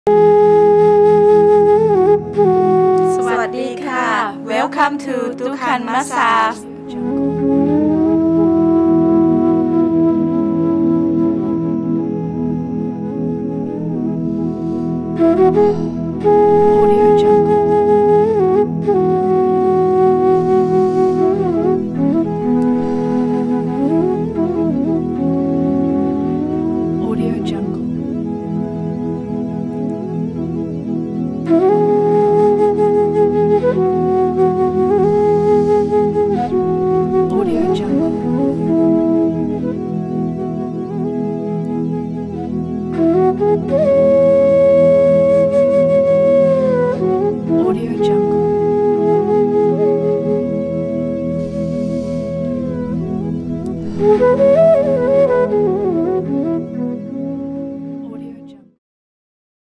Thai Massage @Van der Valk Hotel Gilze-Tilburg
Since this year, Hotel Gilze-Tilburg offers Thai massage as part of its wellness facilities, within the Toucan Health Club. The massages are a treat for the body and are provided by professional masseuses from the famous massage salon Chockdee.